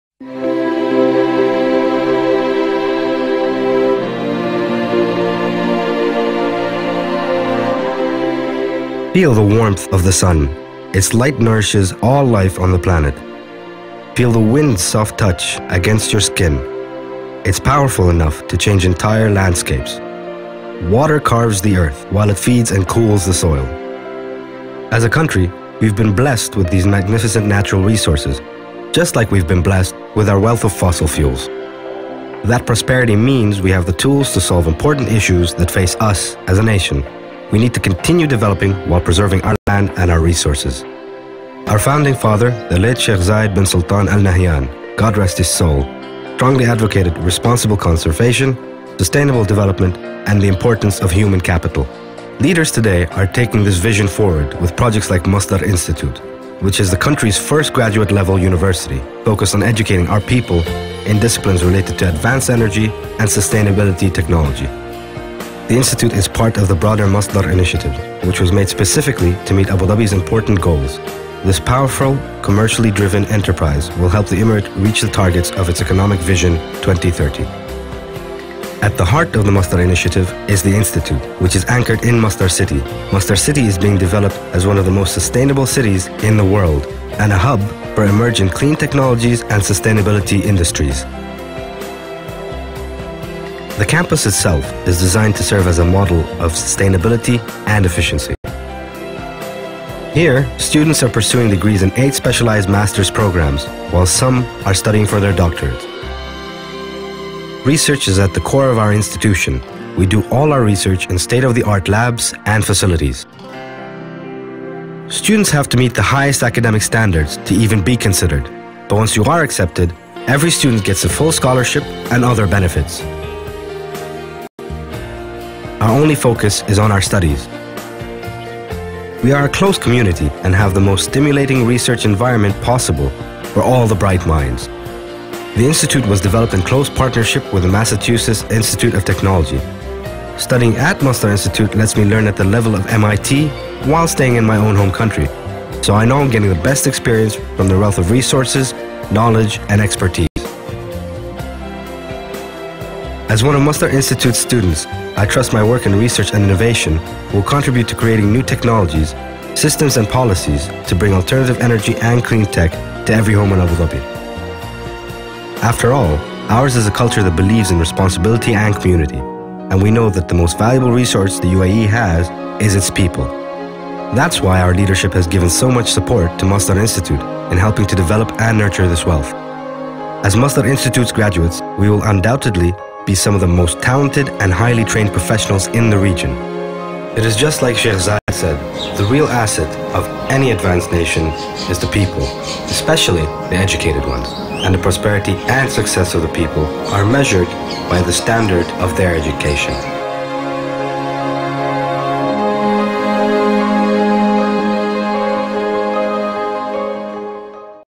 Vision to Reality - Masdar Institute (2013)
Masdar Institute video detailing how Masdar Institute fits into the vision of our Founding Father Sheikh Zayed bin Sultan Al Nahyan. This video also describes the facilities and opportunities available to students who enroll of Masdar Institute.